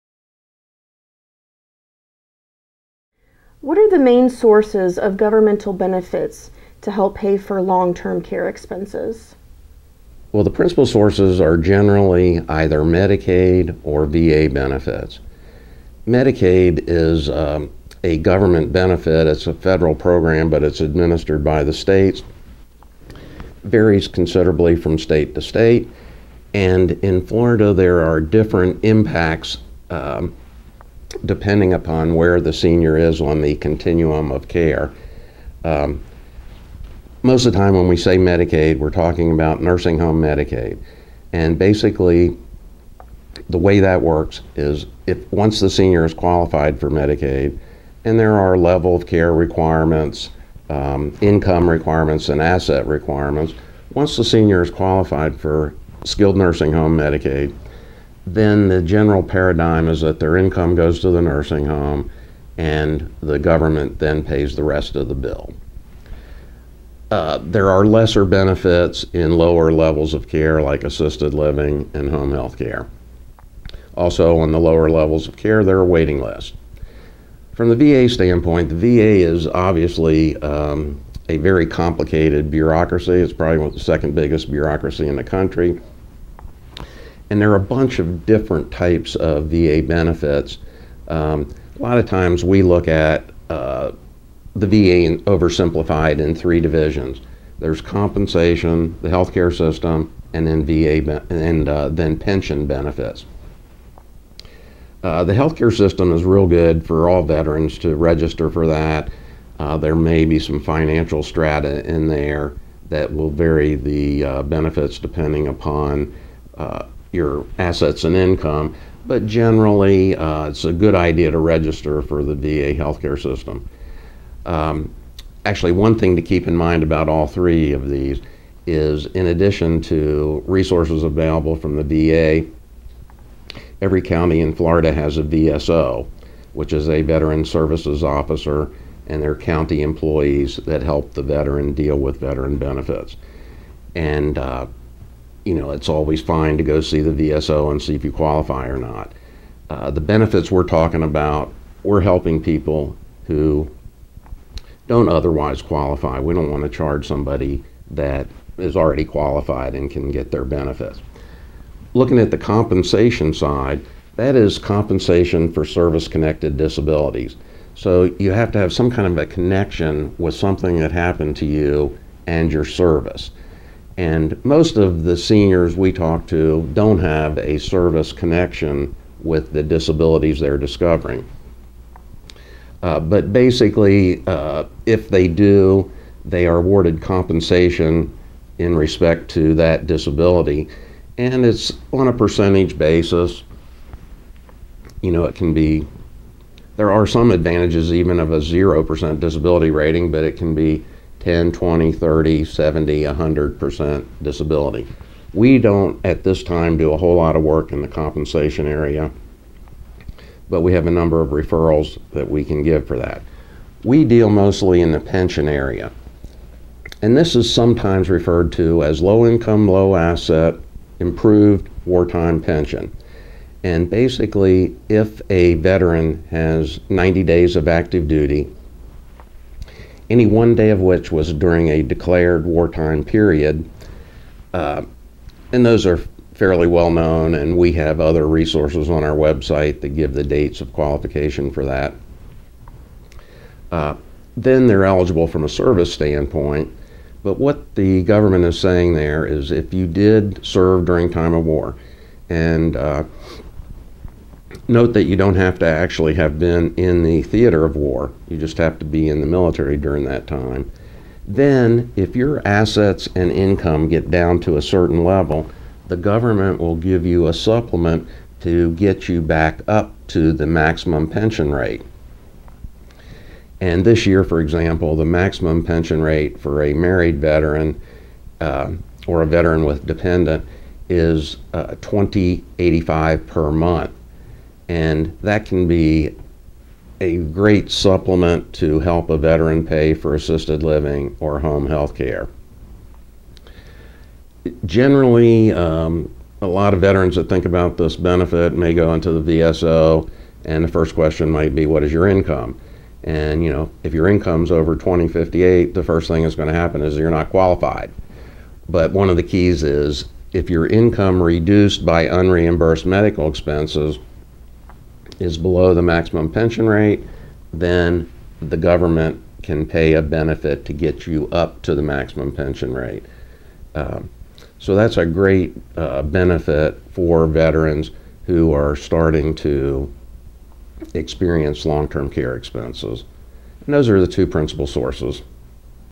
Medicaid Benefits
Attorney Michael D. Fowler answers frequency asked questions on Elder Law, Wills, Trusts and Estates for residents of the Treasure Coast including Vero Beach, Ft. Pierce, Post St. Lucie, Stuart, Palm City and surrounding counties.